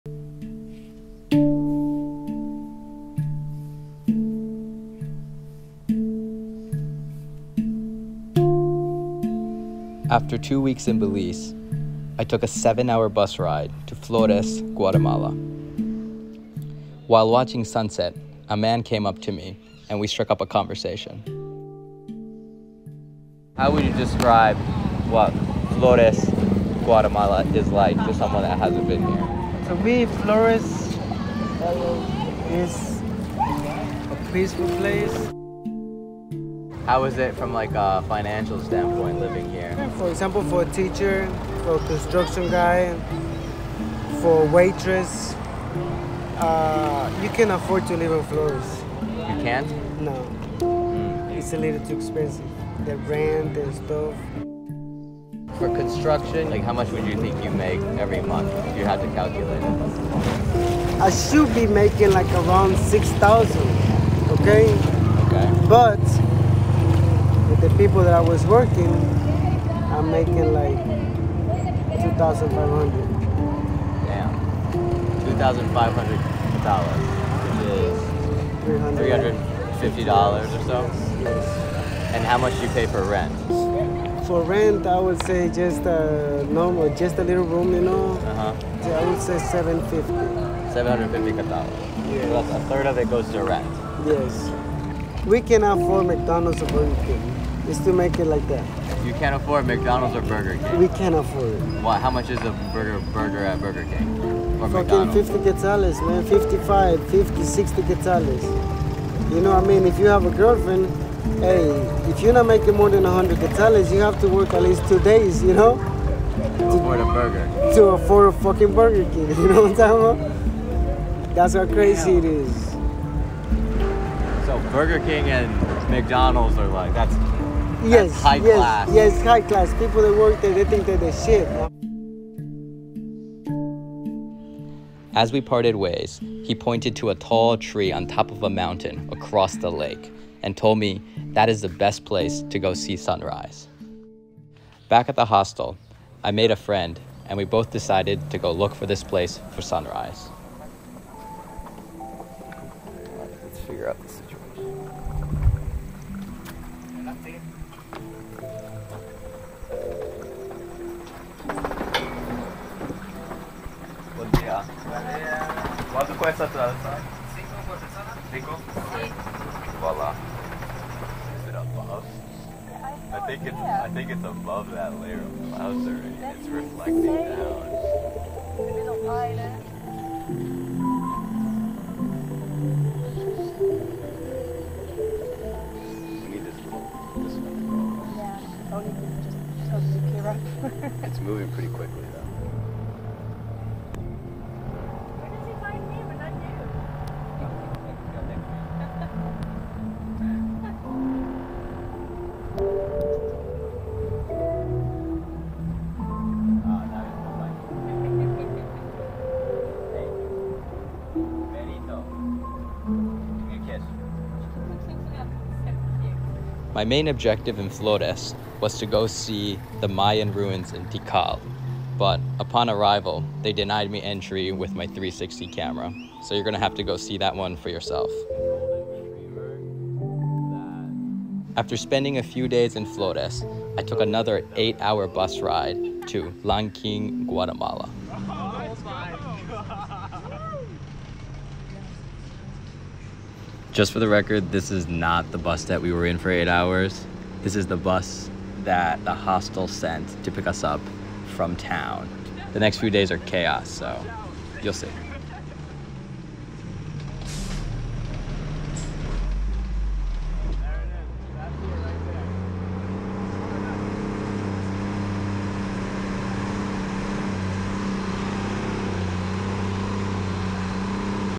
What it’s like to live in Flores, Guatemala - VR 8K360 Episode 3
This is a 360 video, for an immersive virtual reality experience watch with a VR Headset. No headset? No problem. Move your mobile phone around and catch the total 360 experience.

My first stop in Guatemala was Flores, Guatemala. While I was watching sunset, a man came up to me and he shared with me the reality of what it’s like living around Flores, Guatemala.

Experience my time with him in virtual reality. This is a day in my life as a solo-backpacker traveling through Central America.

Action 360 camera
VR Microphone
VR Tripod base
VR Tripod head
V R Tripod head leveler
Wireless Control system for VR camera